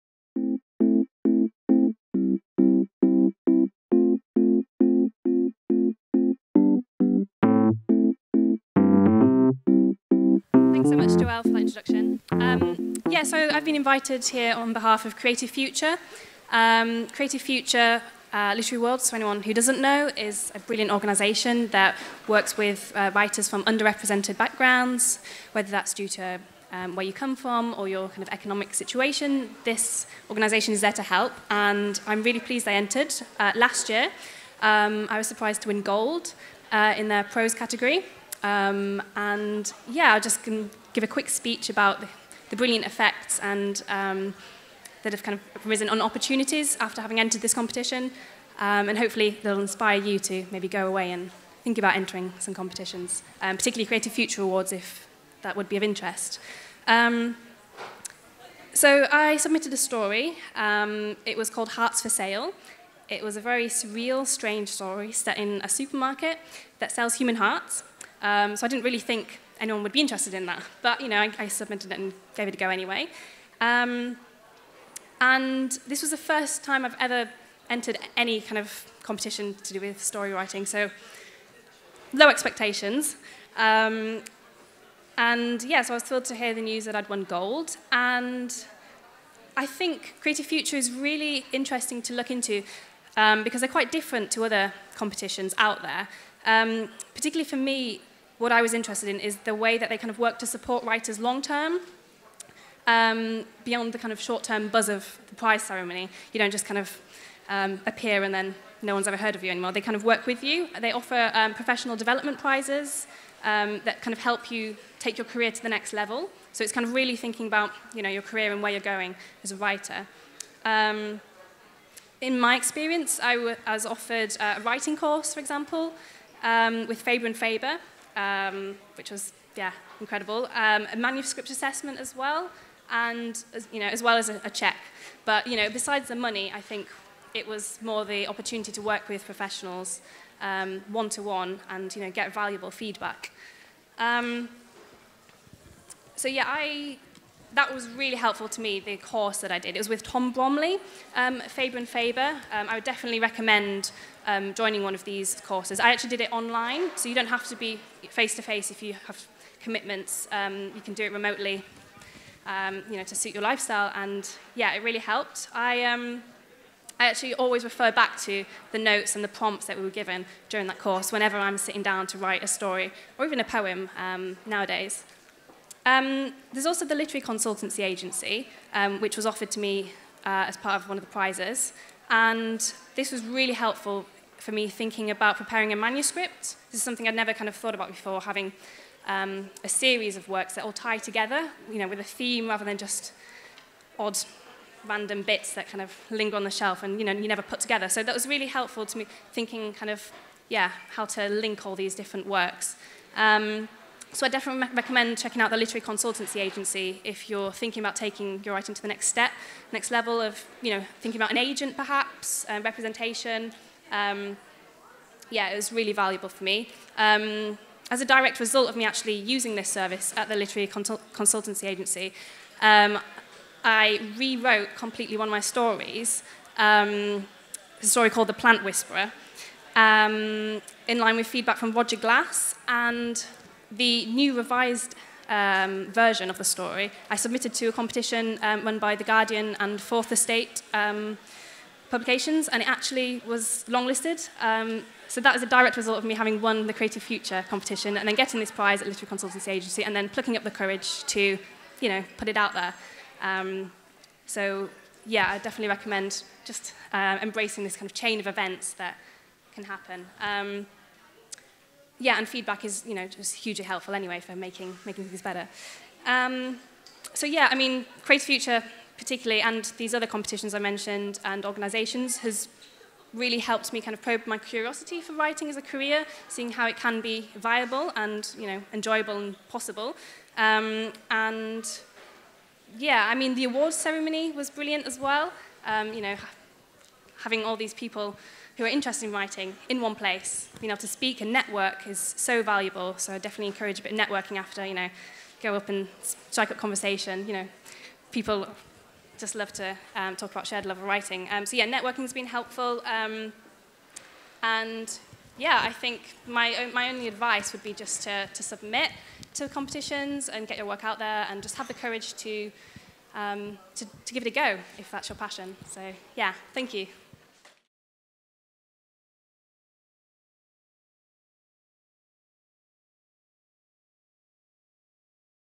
Jade Cuttle on winning a Creative Future Writers’ Award
Writer Jade Cuttle shares her experience of winning a Creative Future Writers' Award and how she's developed her writing career.